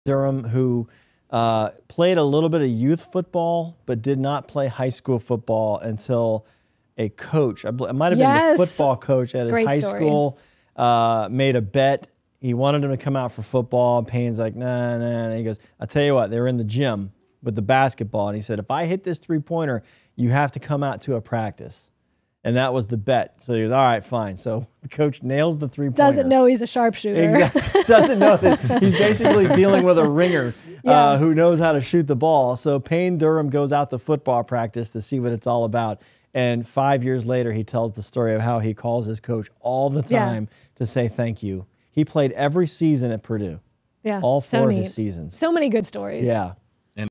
How Bucs rookie tight end got started in football
Bucs Insiders Dan Lucas and Karen Loftus shared the story of how Bucs' rookie tight end Payne Durham got his start in football all because he lost a bet to a former coach.